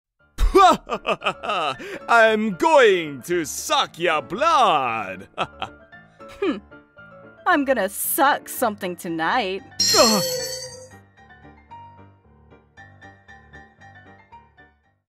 Goten X Marron: Vampire Goten | DBZ Comic Dub
Goten is the vampire but Marron is the one who wants to suck. XD

Voice Of Marron: (mbteddygram)

Voice of Goten: cameron_t

Have you ever seen Goten X Marron: Vampire Goten | DBZ Comic Dub before watching this video?